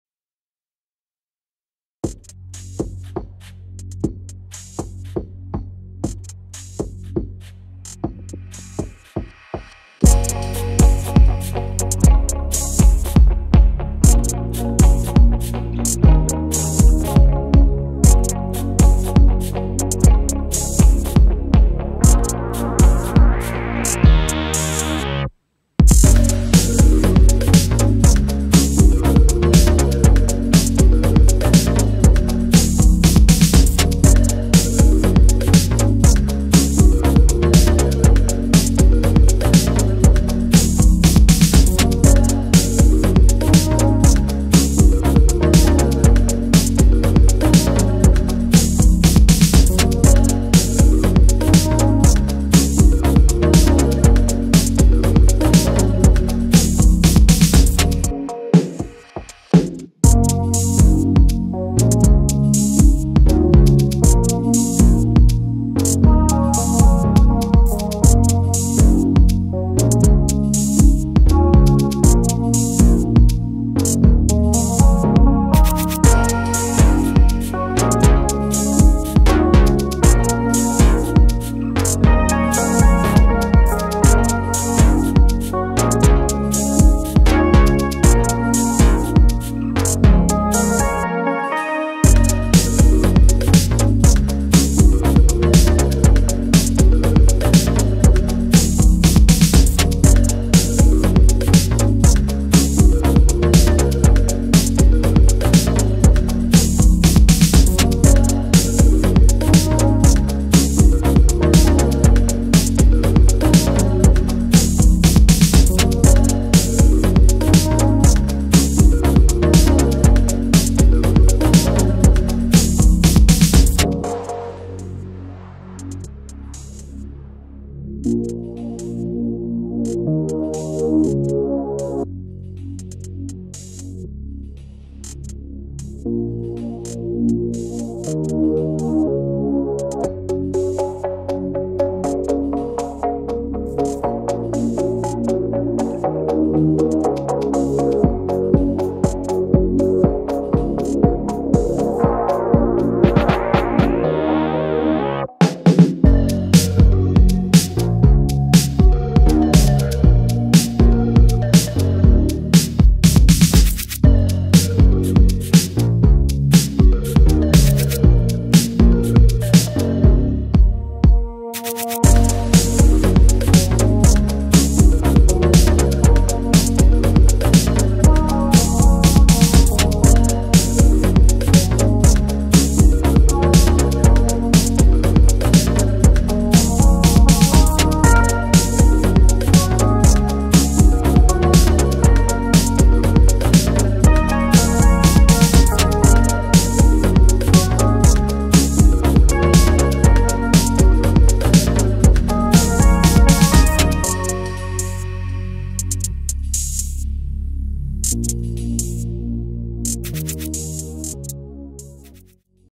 FLEURON en enramada (escala 1:24)
Construcción del navío de 74 cañones, en enramada, FLEURON.